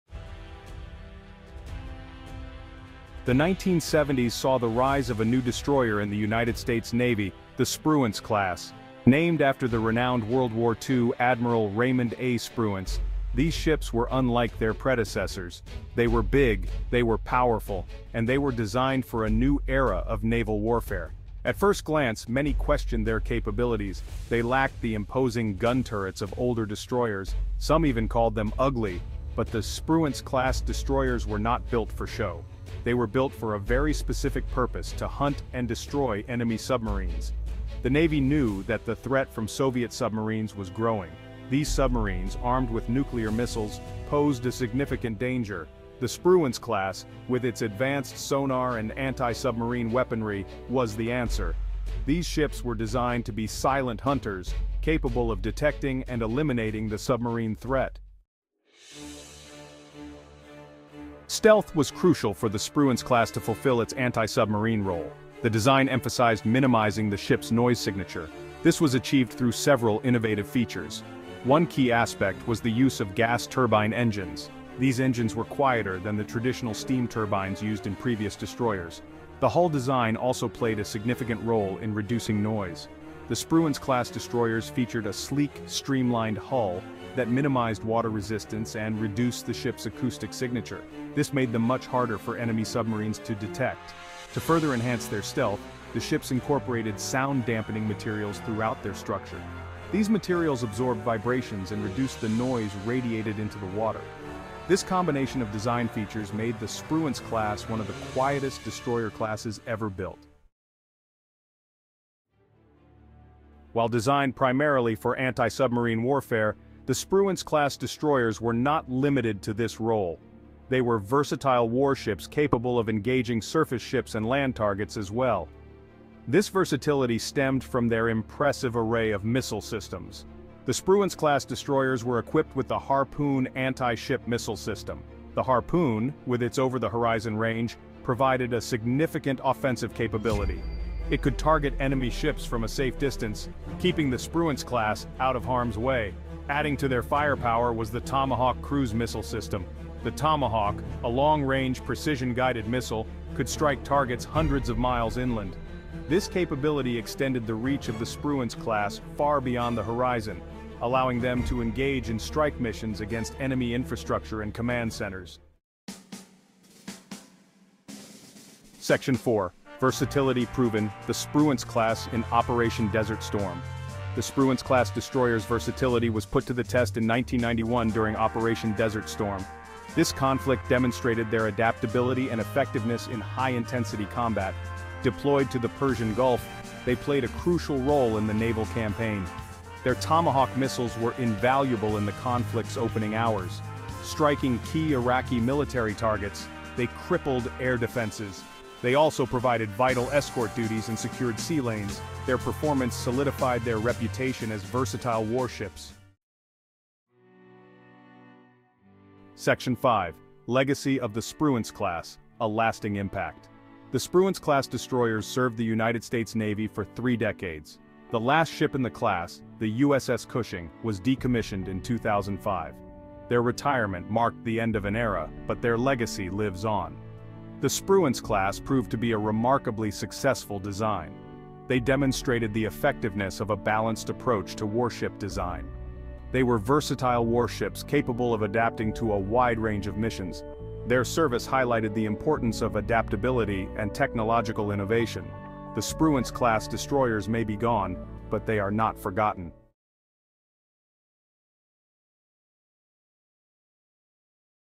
Spruance-Class: This Might Be the U.S. Navy's Best Destroyer Ever
Dive into the fascinating history of the Spruance-class destroyers, named in honor of Admiral Raymond A. Spruance. Designed with a primary focus on anti-submarine warfare, these warships emerged as a critical response to the growing threat posed by Soviet submarines. Learn about the innovative features like quieter gas turbine engines and sound-dampening materials that minimized their noise signature, and how these advancements made them formidable adversaries. Discover their versatility with the Harpoon anti-ship missile system and Tomahawk cruise missiles, extending their operational reach. From their crucial role in the naval campaign against Iraq to their lasting legacy, explore how these destroyers shaped modern naval warfare.

OUTLINE:

00:00:00 A New Breed of Destroyer
00:00:58 Designed for Anti-Submarine Warfare
00:01:50 A Powerful Missile Punch
00:02:45 The Spruance-Class in Operation Desert Storm
00:03:26 A Lasting Impact